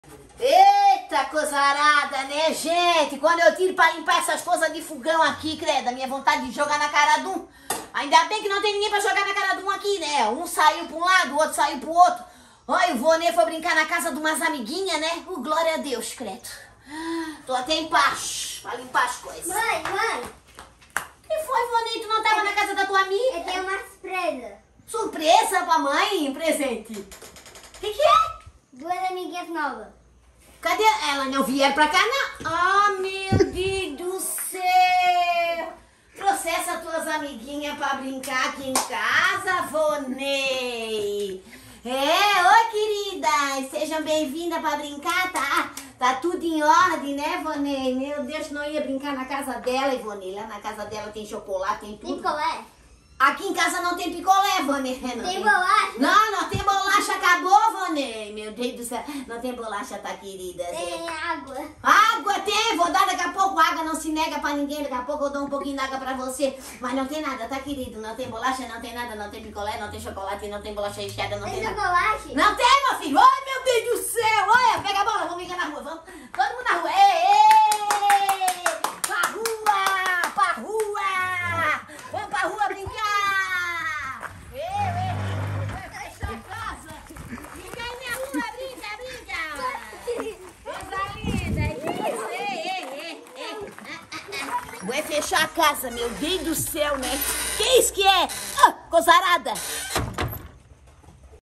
A surpresa do Volney
Hôx só quero ficar em paz, mash o Volney resolveu me fazer uma surpresa, o qui será mô Deus, esse guri é igual o pai, só apronta.

Ixpia e descubra a surpresa do Volney 😂

🎬 Produção
Geral: @murilobonucci
Comercial: @rosangelagarciaassessoria
Digital: @bewaines